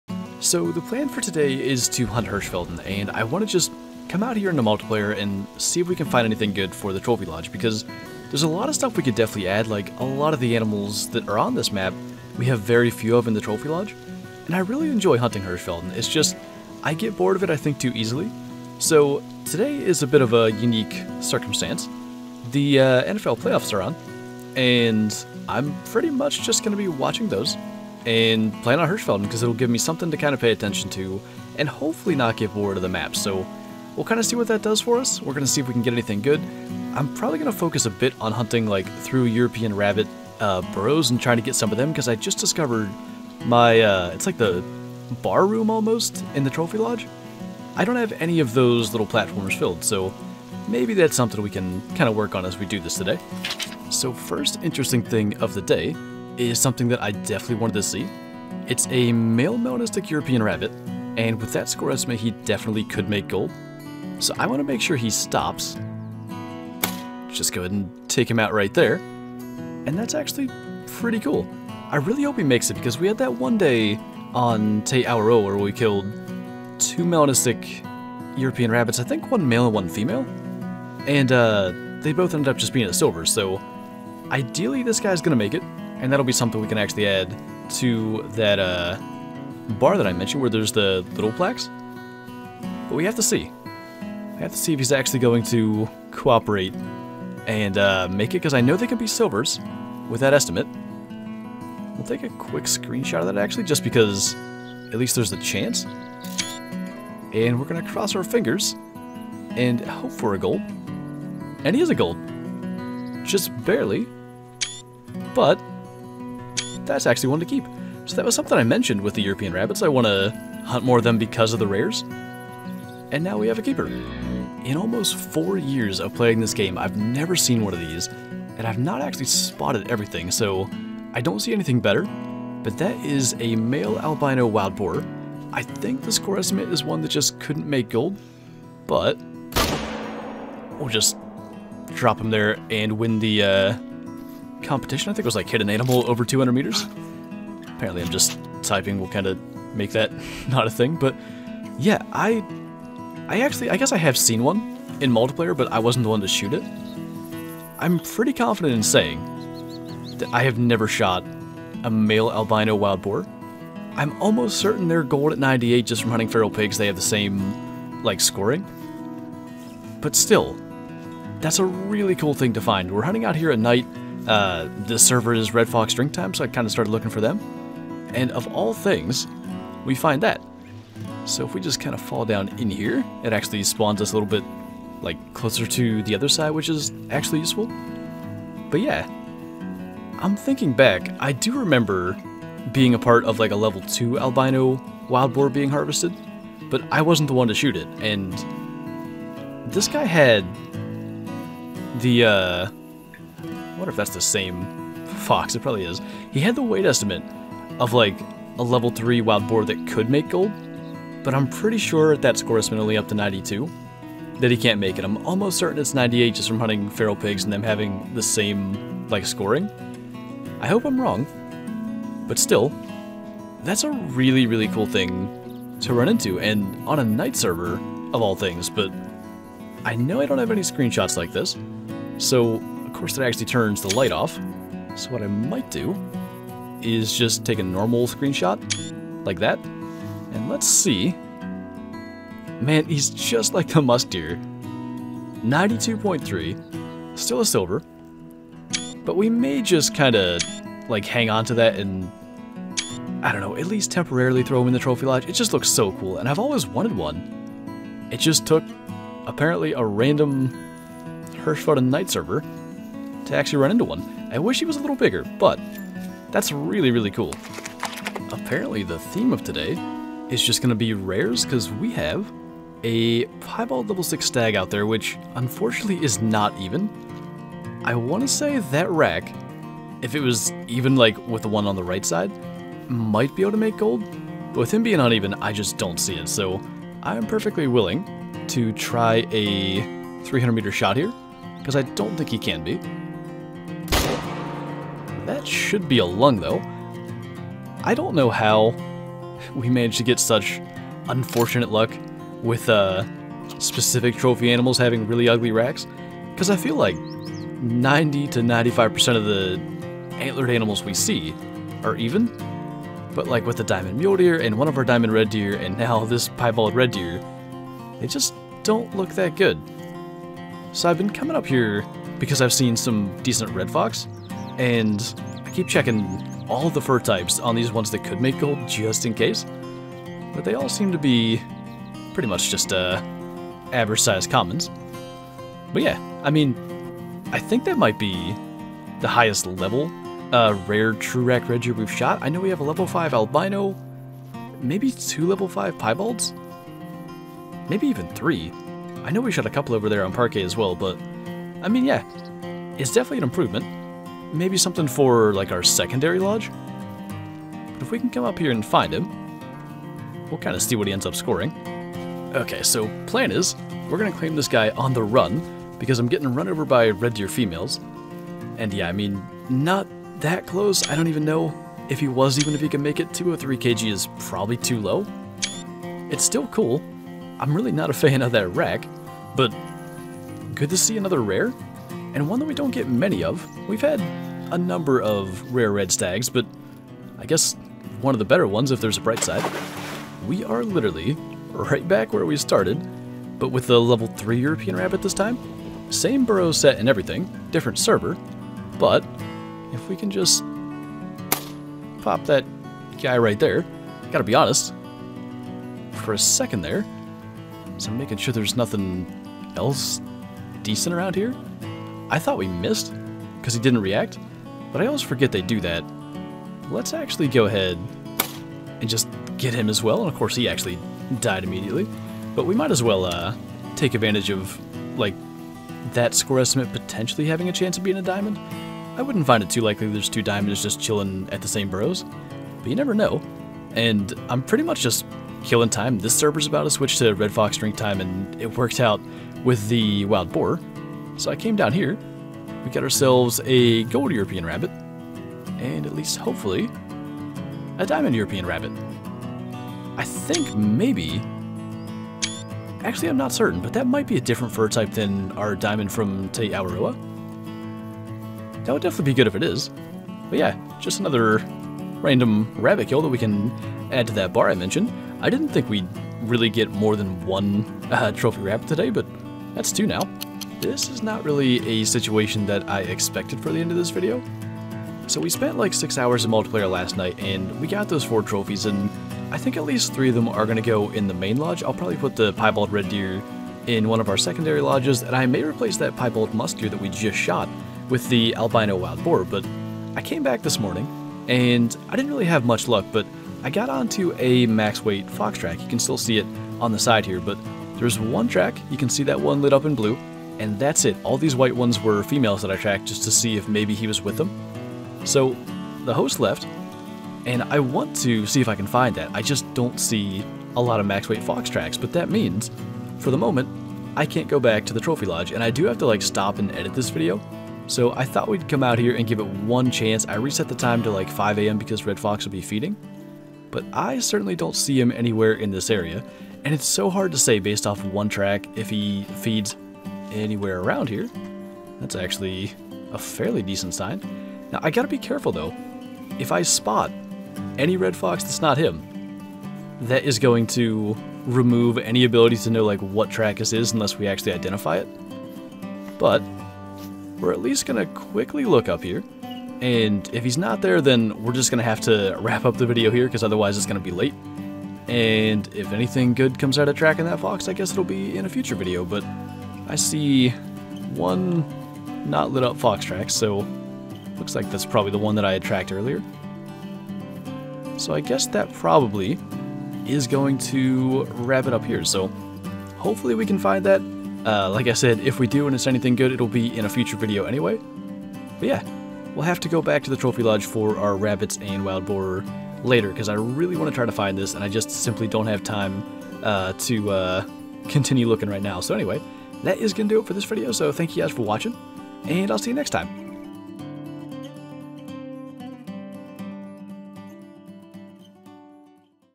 Hunting Hirschfelden Multiplayer Trophies!
Today we head to Hirschfelden in search of trophies for the lodge from multiplayer!

► Special thanks to:
RedneckJay  -  Hamish  -  Mark  -  Jesse  -  Malek  -  Round Tree Ranch  -  Jonathan  -  allstarrider  -  DasBiel  -  Killer Kane  -  Devil Gurr  -  Derrick  -  JaxyBeard